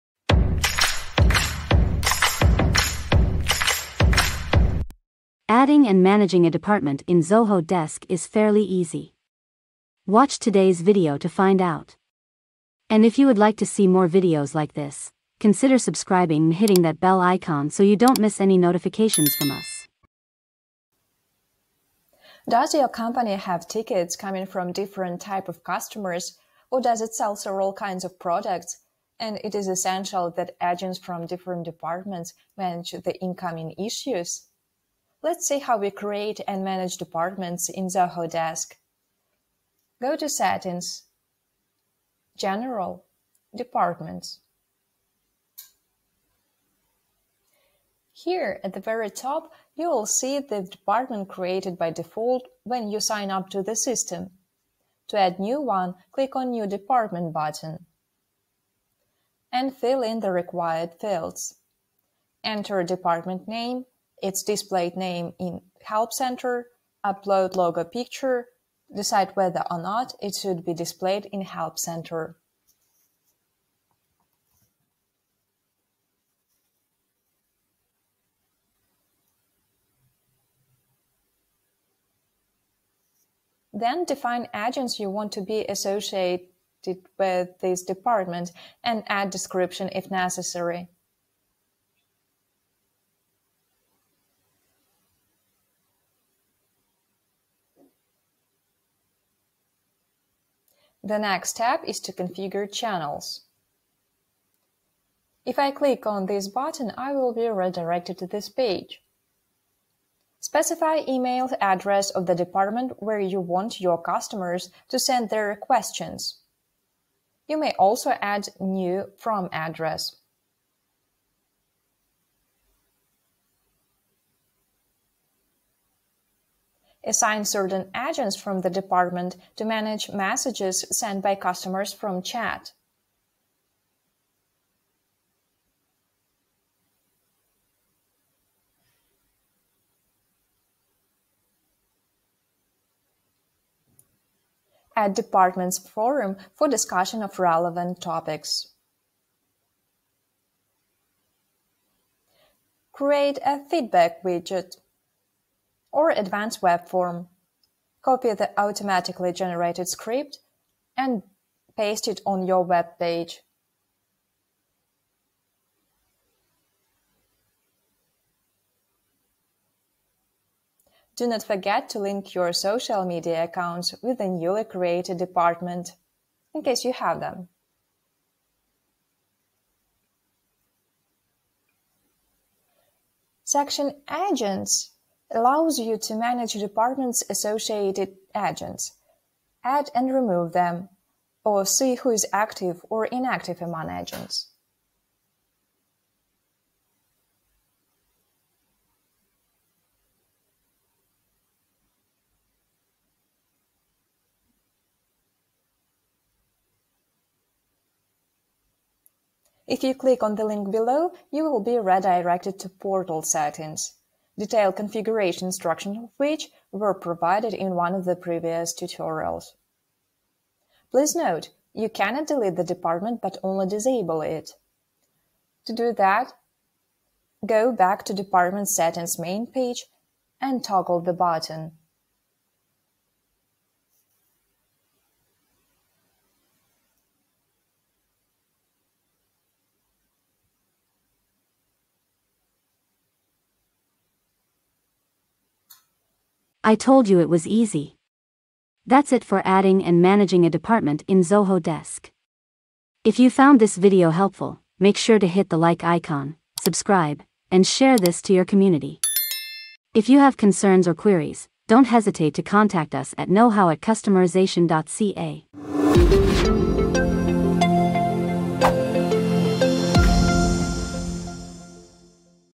How to Add and Manage a Department in Zoho Desk 2022
This video tutorial explains how to manage and create new departments in Zoho Desk.
Does your company have tickets coming from different types of customers? Or does it sell all kinds of products, and it is essential that agents from different departments manage the incoming issues?  Let's see how we create and manage departments in the Zoho desk.

➤ Go to settings, and general departments: here at the very top you'll see the department created by default when you sign up for this system.

To add a new one click on the new department button and fill in the required fields. Enter a department name, and its displayed name in Help Center, upload a logo picture, decide whether or not it should be displayed in the help center, then define agents you want to be associated with these departments, and add a description if necessary.

The next tab is to configure channels: If I click on this button I will be redirected to a page. You may also add a new ‘from’ address, and assign certain agents from the department to manage messages sent by customers from chat.

Add a department forum for discussion of relevant topics. Create a feedback widget or advanced web form. Copy the automatically generated script and paste it on your web page. Do not forget to link your social media accounts with a newly created department if you have them.

Section agents allow you to manage departments associated with agents. Add and remove them, or see who is active or inactive among agents.
If you click on the link below, you will be redirected to portal settings, detailed configuration instructions which were provided in one of the previous tutorials.
Please note you cannot delete the department but only disable it. To do that, go back to the department settings main page and toggle the button.

TIMESTAMPS👀🎬
00:00 Intro
00:55 Add new Department in Zoho Desk
02:25 Add a department forum
03:04 Managing Departments in Zoho Desk

🆘 NEED A CONSULTATION IN ZOHO CRM? 🆘